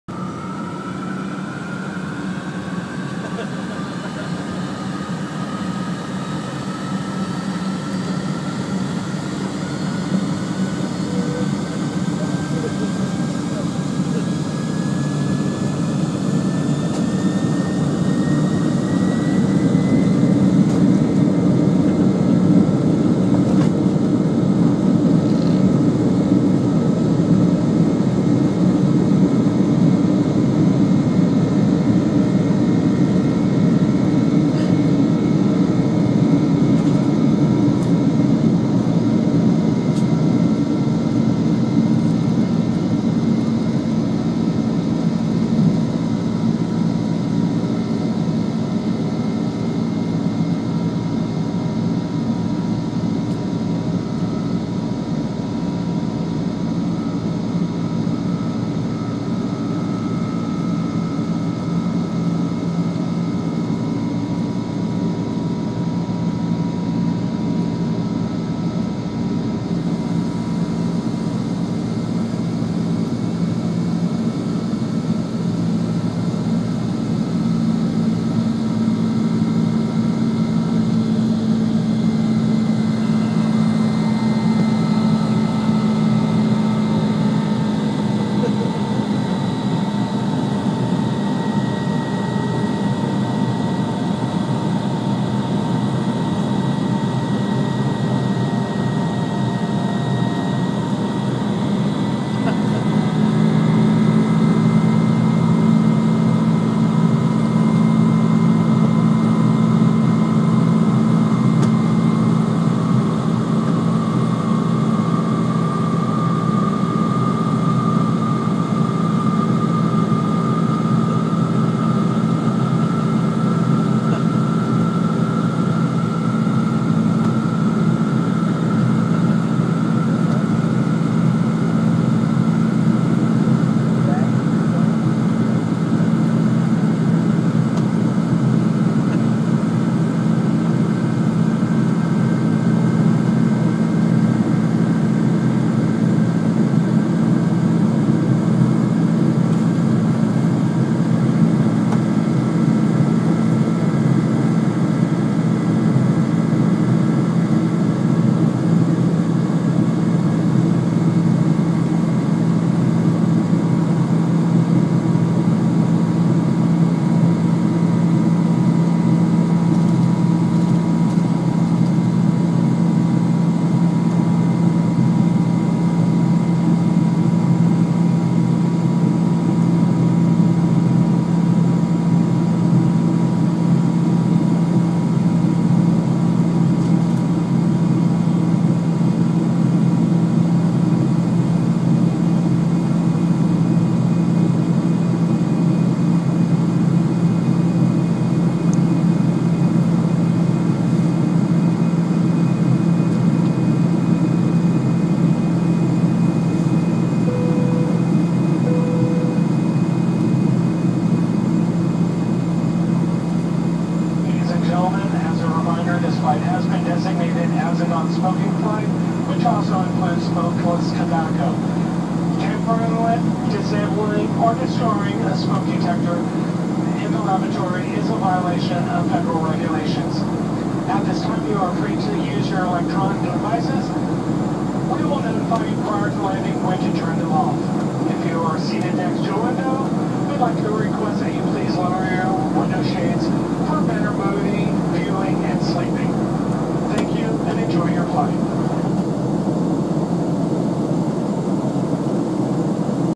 Blasting Through the clouds!!! Fantastic HD MD-11 Takeoff From Gulfport Mississippi!!!
The third leg of my MD-11 journey!  Extremely quick rotation and departure from Runway 32!  Great Pratt & Whitney engine sound!  Low cloud ceilings!  A thunderstorm had recently passed through the area!  Recorded with my iPhone 4S!  Filmed on board World Airways!  Flight to Tinker Air Force Base.  4/3/13.